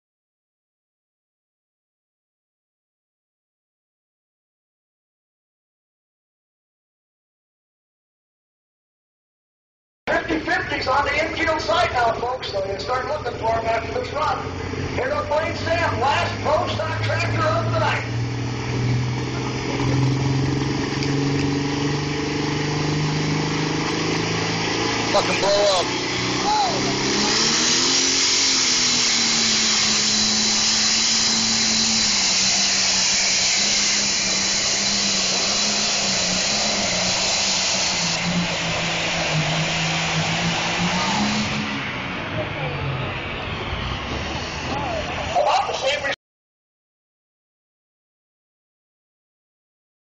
pro stock sled pull rolling coal
pro stock case sled pull at the fair with some nice coal